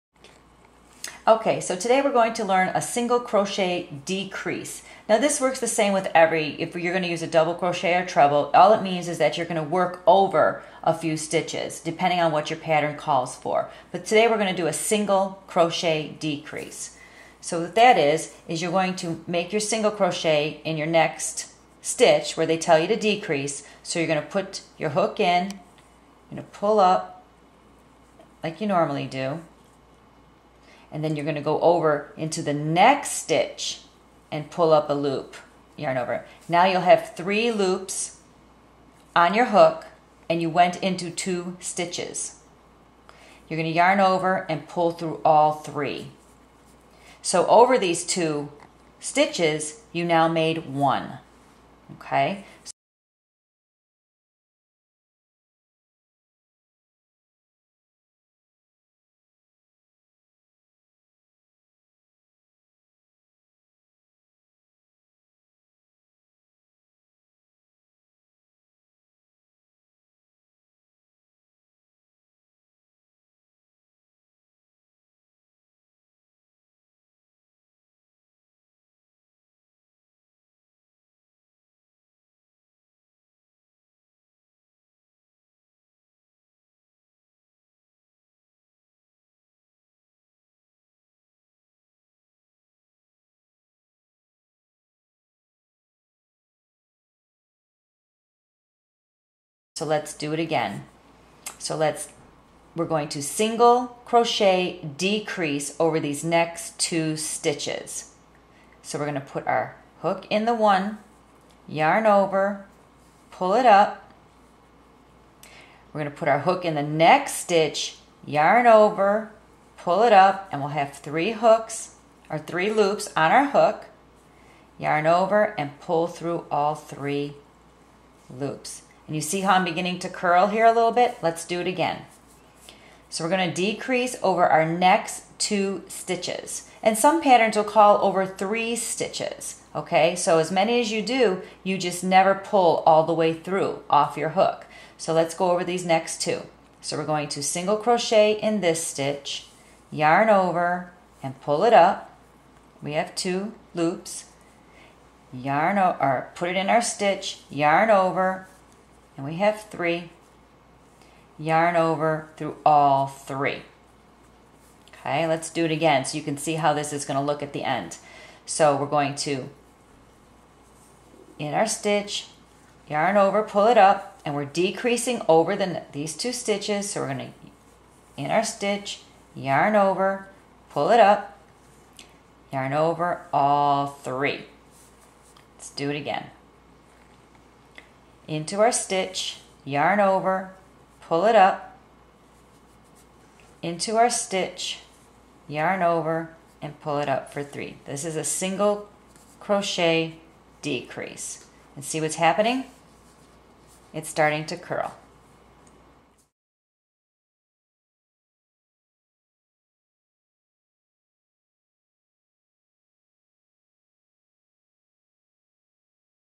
Single Crochet Decrease
Single crochet decrease is not as hard as you think, in this video I will teach you how to do a single crochet decrease.